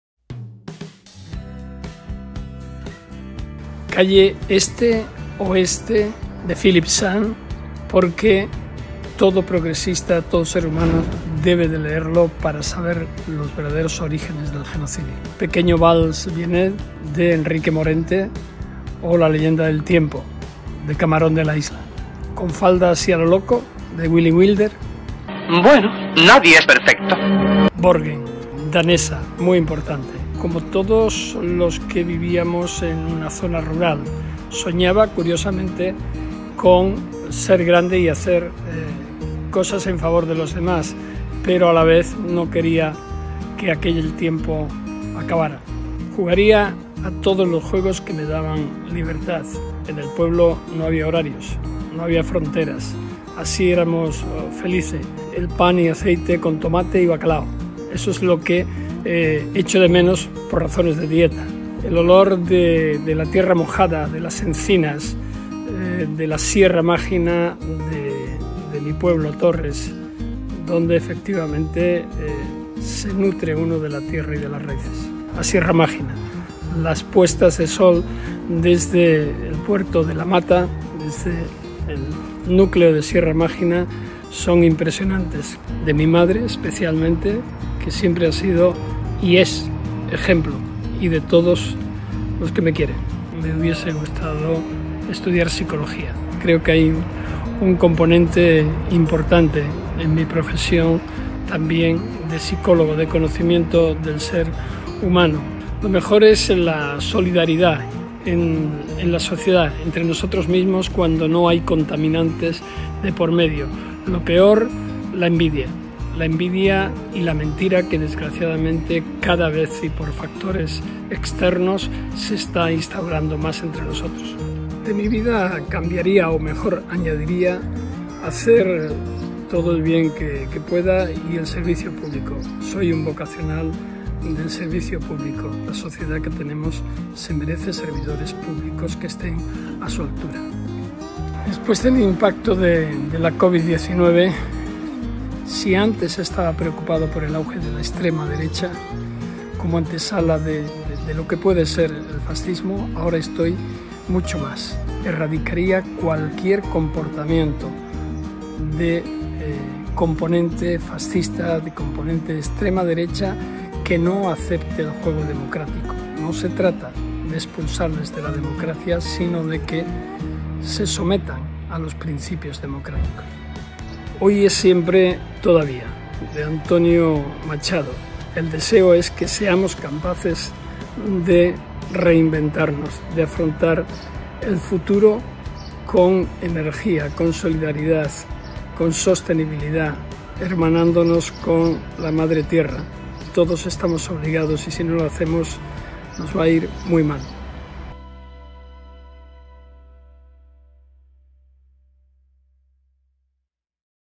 Baltasar Garzón: Erradicaría cualquier comportamiento de componente fascista | La playlist de...
El jurista Baltasar Garzón atiende a infoLibre.
Nos cuenta sus gustos en música , cine y ficción , así como algunas reflexiones personales.